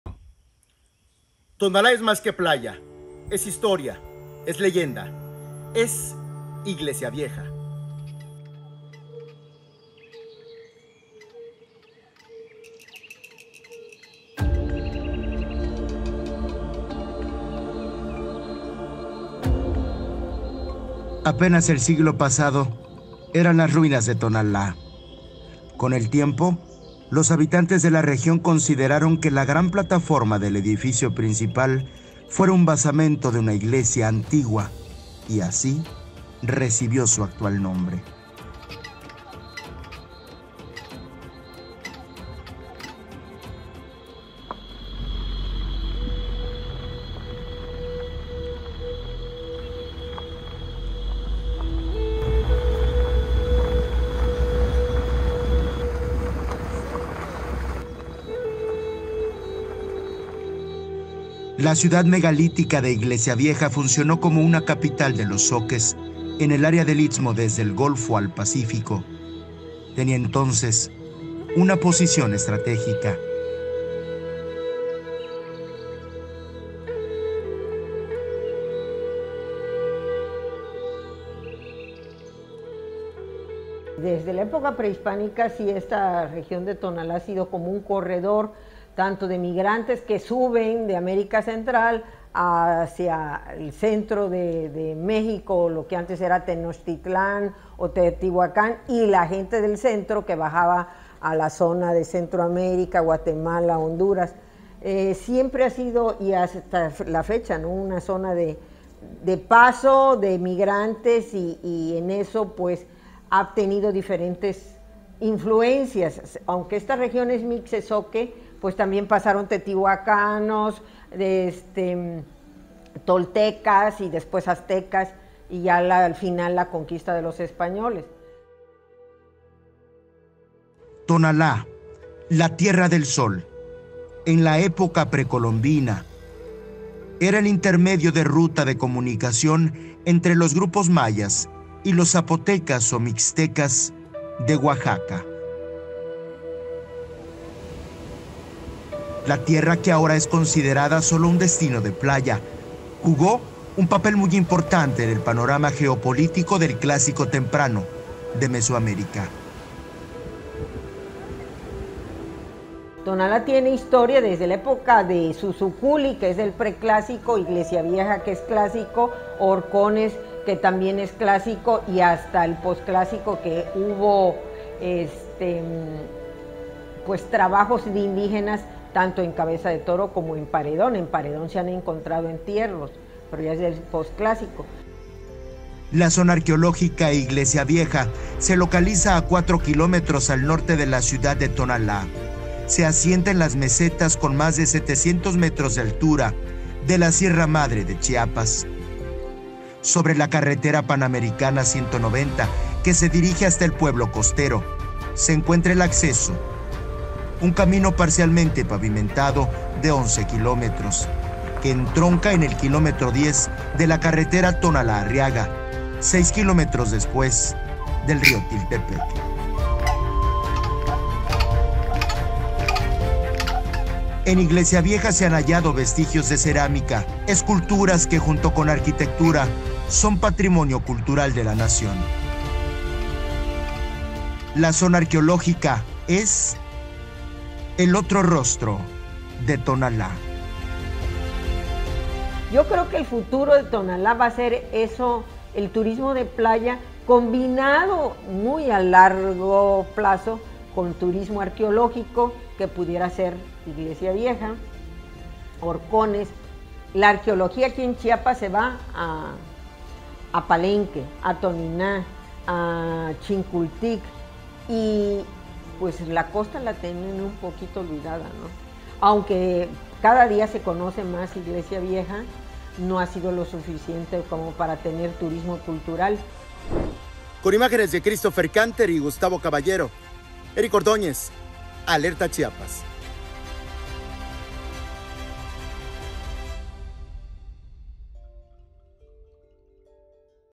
Tonalá es más que playa, es el misterio de 'Iglesia Vieja'
Fue el punto estratégico para pueblos enteros; un siglo atrás eran las “Ruinas de Tonalá”, con el tiempo los habitantes  consideraron que la gran plataforma del edificio principal fuera un basamento de una iglesia antigua y así recibió su actual nombre.